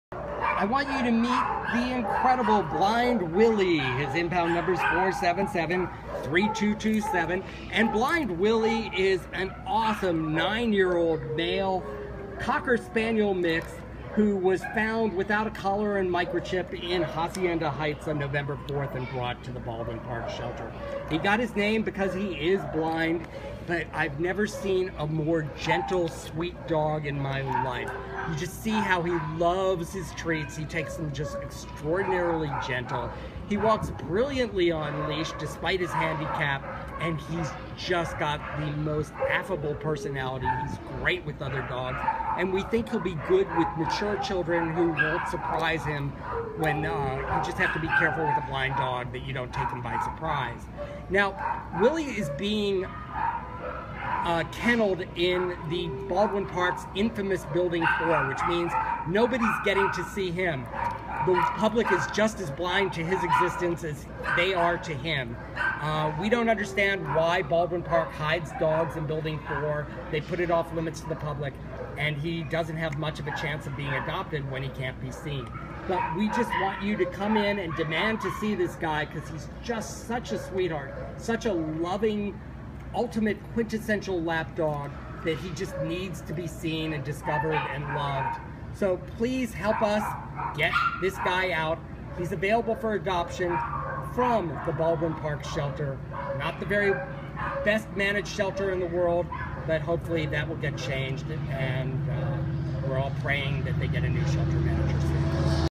Blind Willie A4773227
A4773227  Blind Willie is an affable blind nine year old red with white markings Cocker Spaniel mix who was found without a collar or a microchip in Hacienda Heights on November 4th and brought to the Baldwin Park Shelter where he currently is being hidden out of view of the public in the off limits Building 4.  Weighing seventeen pounds, Blind Willie is a calm, low energy dog who despite being blind, walks capably on leash.  Tolerant of other dogs, we think Willie would be happiest in a calm environment where he could do what he does best – sitting on the sofa being a loving lapdog, taking treats gently and acting as a couch potato!  Blind Willie is a phenomenal companion for a senior citizen as well as an excellent indoor pet for an individual or mature family in any living arrangement.